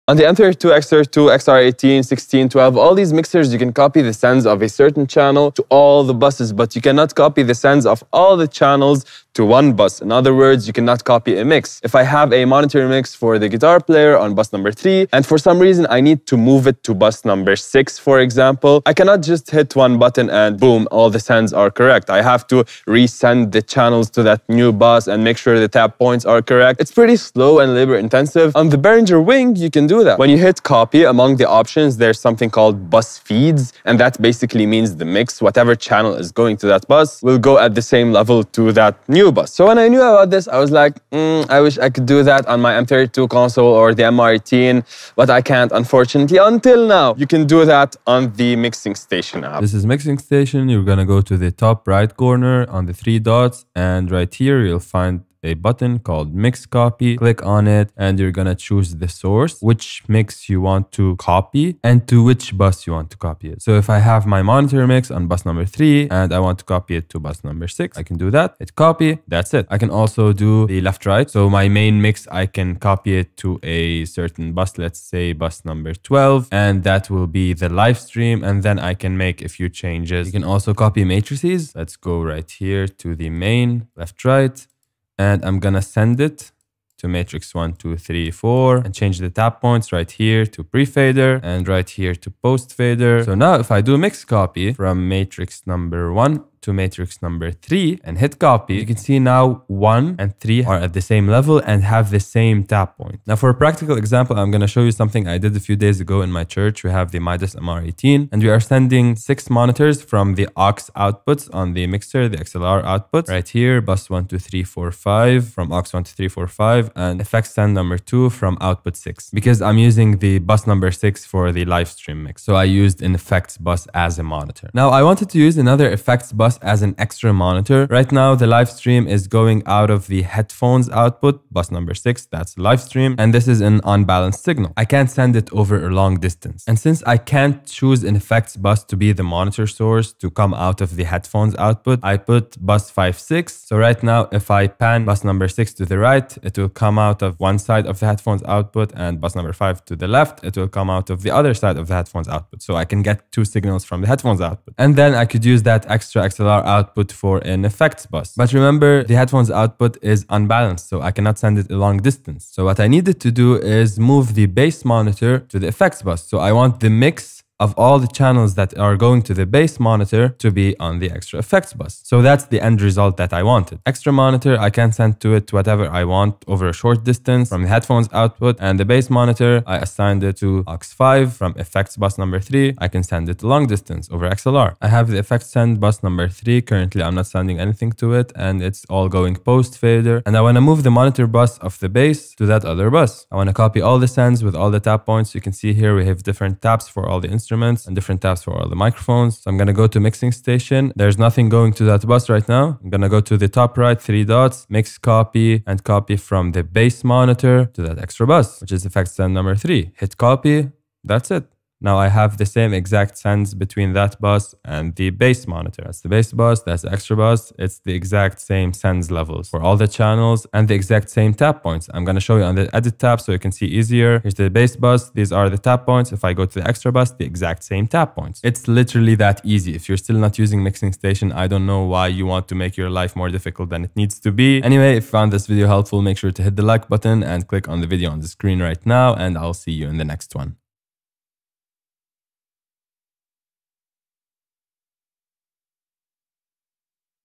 Copy monitor Sends on X32 & XR18/16/12 | Mixing Station: mix copy
how to copy the sends from a mixbus to another in mixing station for midas m32 & behringer x32 & x air mixers